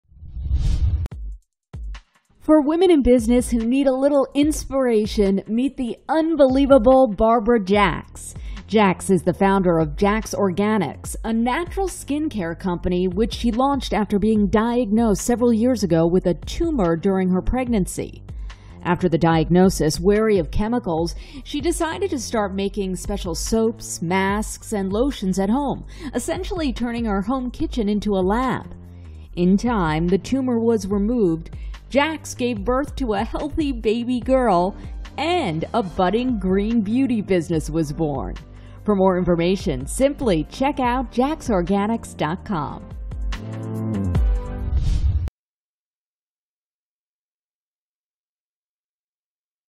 An Inspirational Beauty Business
For women in business who need a little inspiration, meet the unbelievable Barbara Jacques. Jacques is the founder of Jacq’s Organics, a natural skin-care company which she launched after being diagnosed with a tumor during pregnancy. After the diagnosis, wary of chemicals, she decided to start making her soaps, masks and lotions at home — essentially turning her home kitchen into a lab. The rest is history…